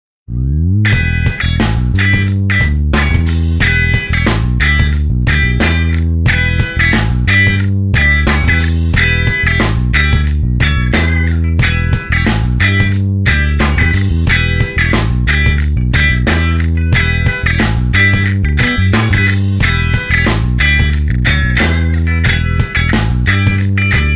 Animation eines 3D Puzzles
Diese Puzzle stammt aus den 80er Jahren
und ich habe es tatsächlich mal besessen.
Weil ich es hübsch fand, habe ich diese Animation davon erstellt.